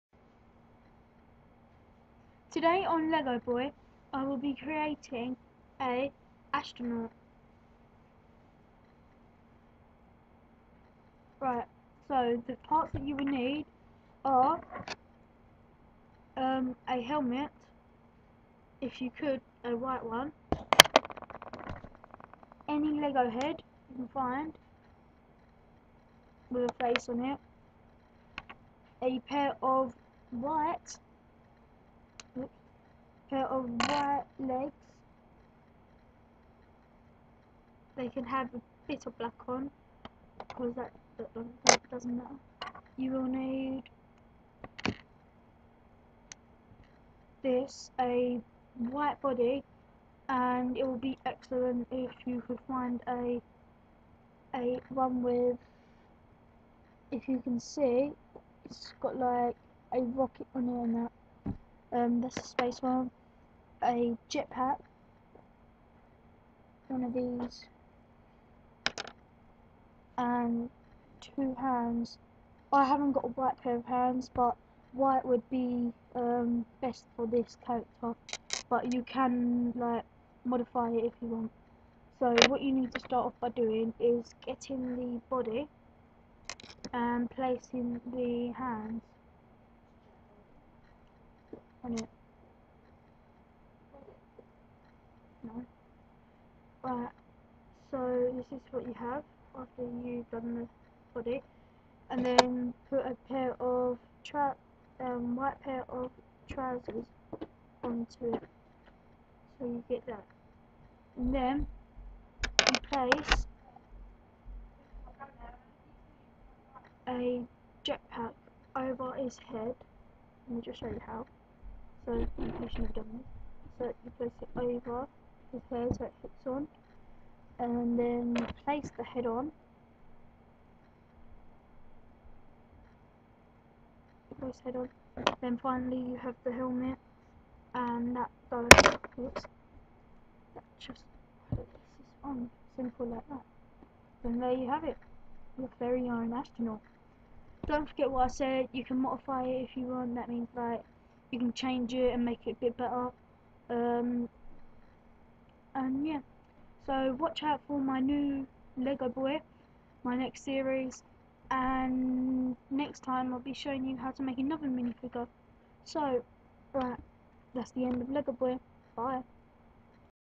LEGO boy ( astronaut )
joe keenan ( me ) making a astronaut.